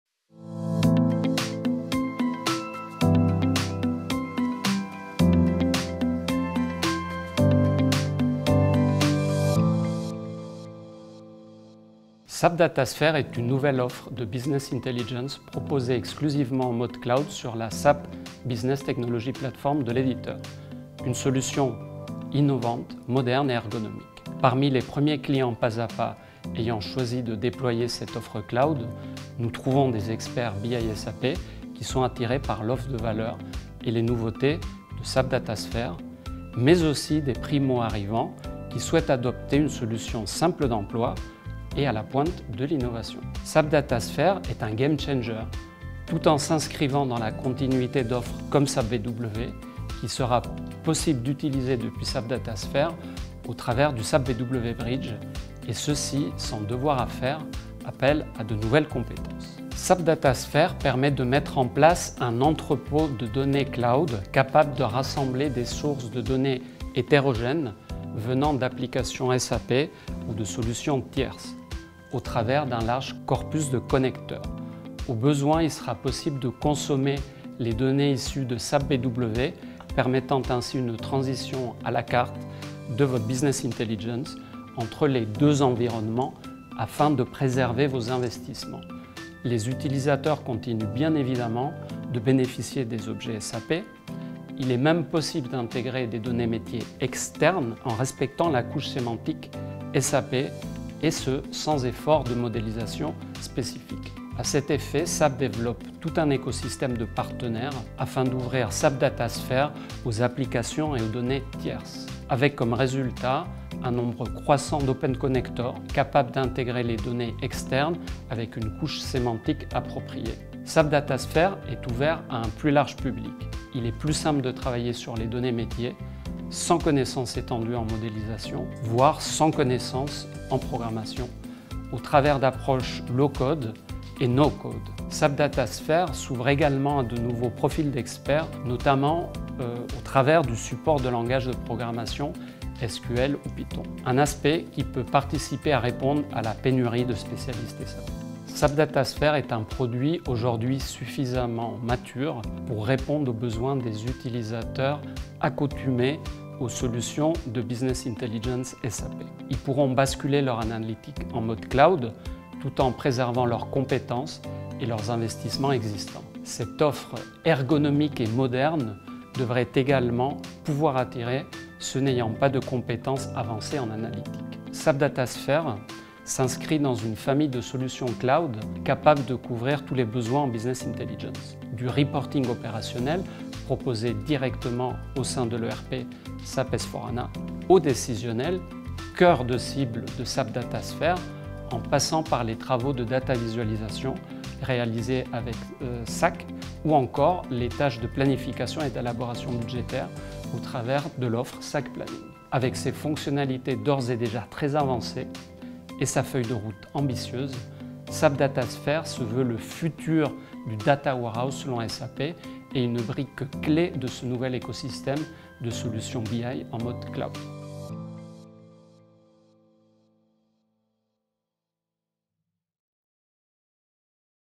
SAP Datasphere : offre analytique full cloud
SAP Datasphere permet de mettre en place un entrepôt de données cloud capable de rassembler des sources de données hétérogènes, venant d’applications SAP ou de  solutions tierces, au travers d’un large corpus de connecteurs.

Raphaël Hevia, Architecte BI chez PASàPAS, nous en dit plus sur les raisons de choisir cette solution.